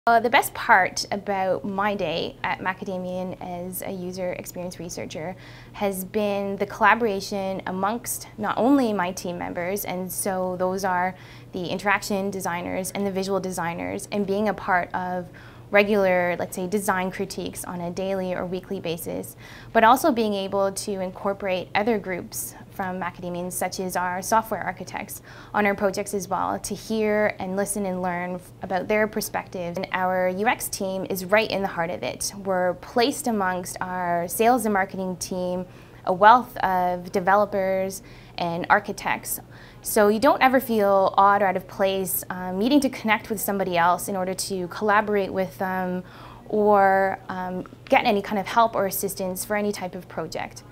Why we love Macadamian- Collaborating
Anneliis Tosine, User Experience Researcher, talks about Macadamian using collaboration as a work tool.